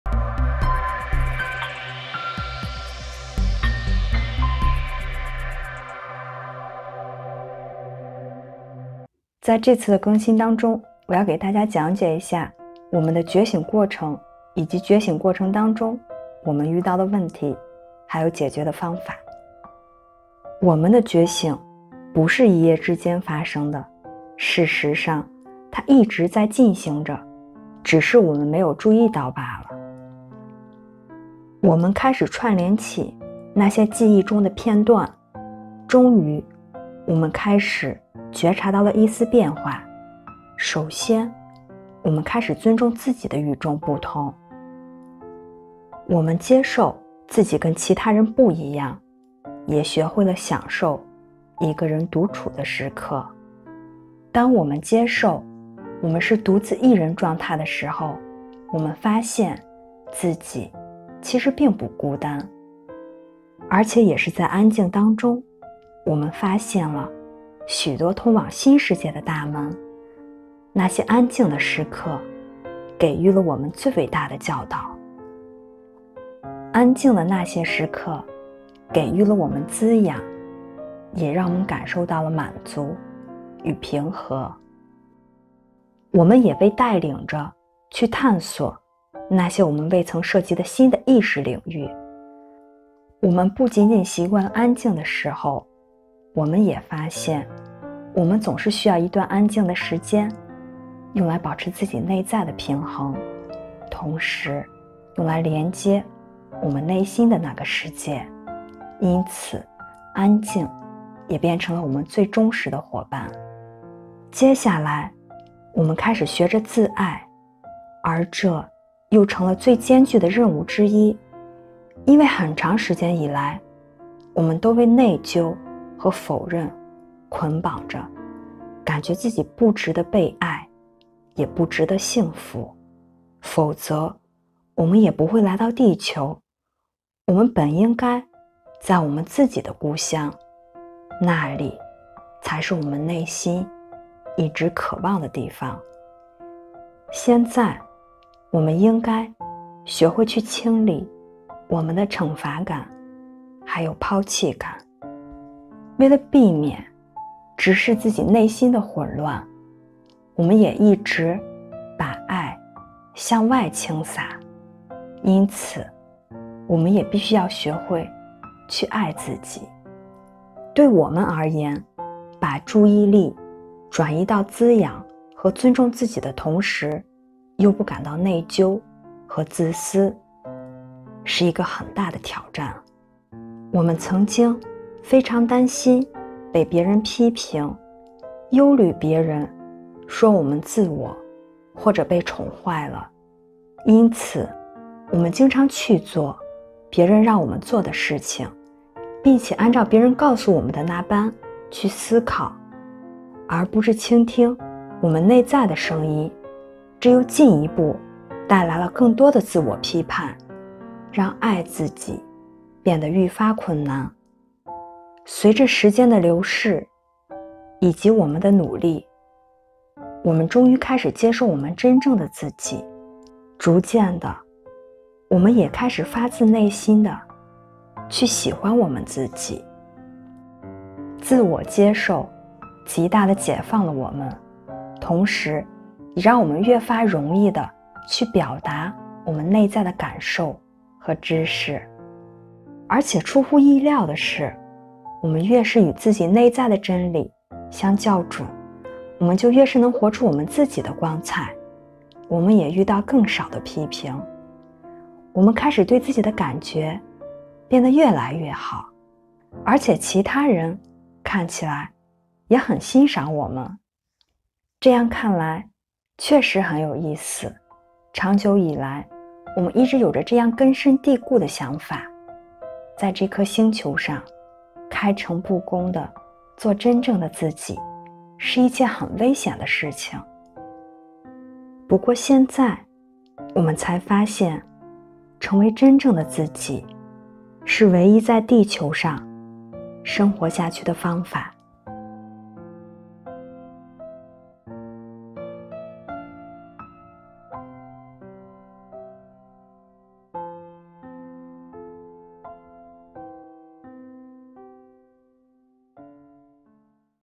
Eng sub【觉醒 The Awakening】在地球上活出真正的自己 Openly Be Ourselves on Planet Earth
The awakening did not take place overnight. Actually, it had been happening all along. Only we had not recognized it.
我们的觉醒不是一夜之间发生的，事实上，它一直在发生着，只是我们并没有注意罢了。

Remember that we had been gathering and stringing our pearls of remembrance since the beginning. Eventually, we began to perceive a change. First, we started to honor our uniqueness. We embraced the fact that we were different and learned to enjoy our sense of aloneness.

终于，我们开始觉察到一丝变化。首先，我们开始尊重自己的与众不同。我们接受自己跟其他人不一样，也学会了享受一个人独处的时刻。

As we accepted being alone, we discovered that we need not be lonely. In our solitude, we learned to enter the mysterious Zone of Silence. Here we found the key to many new worlds of revelation.
当我们接受自己是独自一人的状态时，我们又发现，我们并不孤独。也是在安静之中，我们发现了通往许多新世界的大门。
Silence proved to be one of our greatest teachers.
那些安静的时刻，给予了我们最伟大的教导。
It embraced us, wrapping us in a profound sense of contentment and peace. We were initiated into new levels of awareness which had previously been unknown to us. We not only became used to the silence, but found that we required steady doses of it to maintain our inner equilibrium and connection to the ONE. Silence became one of our truest allies.
安静的那些时刻，给予了我们滋养。让我们感到满足与平和。我们也被带领着去探索那些我们未曾涉及的新的意识领域。我们不仅仅习惯了安静的时候，同时也发现，我们总是需要一段安静的时间来保持我们内在的平衡，用来连接我们内在的那个世界。安静，成为了我们最真实的伙伴之一。
Next, we started working on self-love. This proved to be one of the most formidable tasks of all, for long had we been locked into guilt and denial, feeling unworthy of love and happiness, or else we would not have found ourselves on this planet at all. We would have been home where we belonged, where we had always yearned to be. Thus, we had to learn to relinquish our sense of punishment and abandonment.
接下来，我们开始学着自爱。而这又成了最艰巨的任务之一，因为很长时间以来我们都被内疚和否认捆绑着，感觉自己不值得爱与幸福，否则我们也不会来到地球。我们本应该在我们自己的故乡，那里才是我们内心一直渴望的地方。正因如此，我们才应该学会放弃我们的惩罚感和抛弃感。
Instead of constantly pouring out love to others as a distraction to avoid facing our own internal turmoil, we had to learn to love ourselves.
为了避免直面内心的混乱，我们一直把自己的爱向外倾洒。我们必须去学会爱自己

Turning our attention to nourishing and honoring ourselves without guilt, without feeling selfish, was a major challenge.
对我们而言，把注意力转移到滋养和尊重自己的同时，又不感到内疚和自私，是一个很大的挑战。
We were so afraid of being judged harshly by others as egocentric or spoiled, that usually we did what people told us to do and thought what they told us to think, instead of listening to what we knew to be true. This caused more self-judgement, making it even more difficult to love ourselves. With the passing of time and much effort on our parts, we finally began to accept ourselves. Gradually, we started genuinely liking ourselves.
我们曾经非常担心被别人批评，忧虑别人说我们自我，或者被宠坏了，因此我们经常去做别人让我们做的事情，并且按照别人告诉我们的那般去思考。而不是倾听那些我们所知道的真相。而这又进一步带来更多的自我评判，让爱自己变得愈发困难。随着时间的流逝，以及我们的努力，我们终于开始接受我们真正的自己。逐渐地，我们也开始发自内心地去喜欢我们自己。

Self acceptance was so tremendously liberating that it became increasingly easier to express our true feelings and inner knowing. And surprisingly enough, the more aligned we were to our deepest Truth and the more openly we lived our Truth––the less criticism did we encounter. Not only were we beginning to feel good about ourselves, but others seemed to appreciate us more too!
自我接受极大地解放了我们，让我们愈发容易地表达我们内在的感受和知识。而且出乎意料的是，我们越是与自己内在的真理相校准，我们就越是能活出我们的光彩 — 我们也遇到更少的评判。我们开始对自己的感觉变得越来越好，而且其他人看起来也很欣赏我们！

It's funny how long we held fast to the judgement that it was dangerous to openly be ourselves on this planet. Now, we are finally discovering that it is the only safe way to be.
这样看来，确实很有意思，长久以来我们一直有着这样根深蒂固的想法：在这颗星球上，开诚布公地去做自己，是一件危险的事情。现在，我们才发现，成为真正的自己，是唯一安全的在地球上生活的方法。